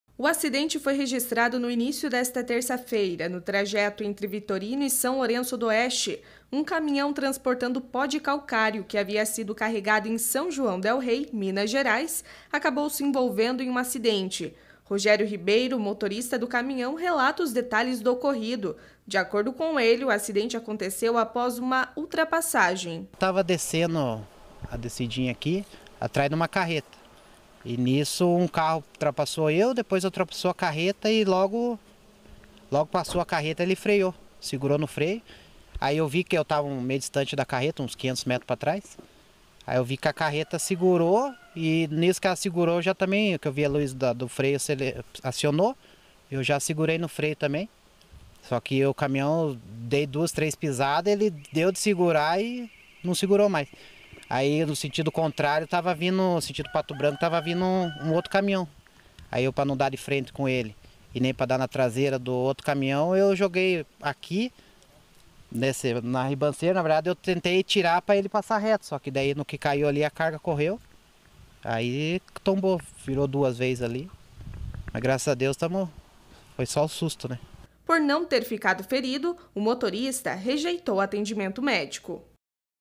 Caminhão despenca em riacho na PR-158
Segurança: Um acidente ocorreu nesta terça-feira (05) na PR-158, quando um caminhão despencou em um riacho. Apesar da seriedade da situação, o motorista saiu ileso.